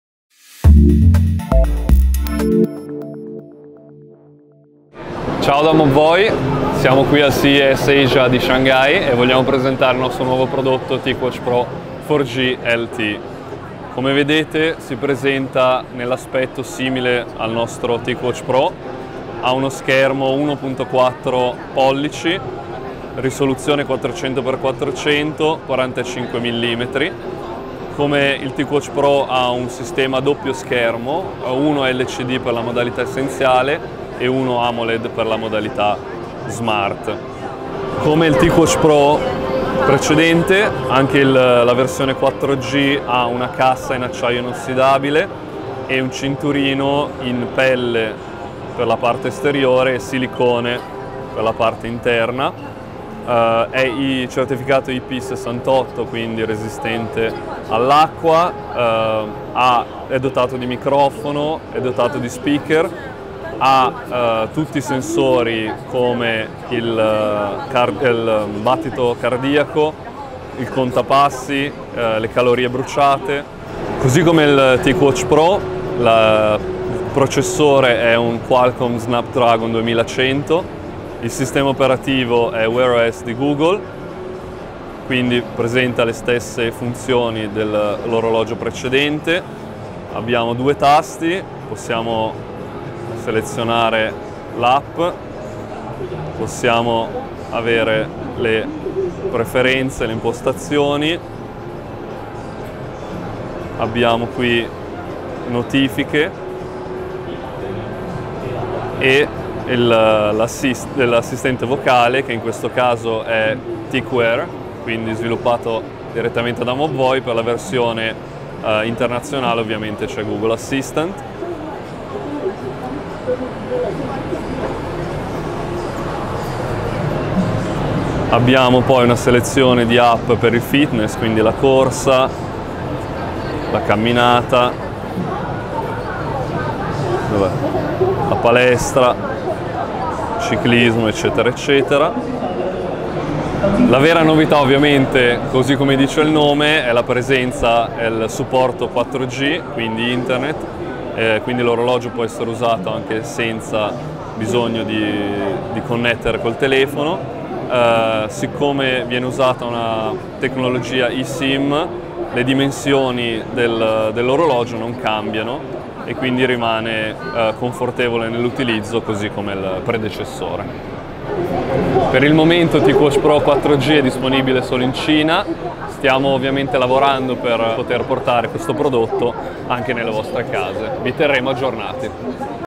Mobvoi Ticwatch Pro 4G ITA
Abbiamo messo le mani in anteprima su TicWatch Pro 4G, lo smartwatch di Mobvoi identico al TicWatch Pro (Bluetooth) presentato un anno fa, sia dal punto di vista estetico che tecnico, con in più connettività 4G-LTE tramite eSIM.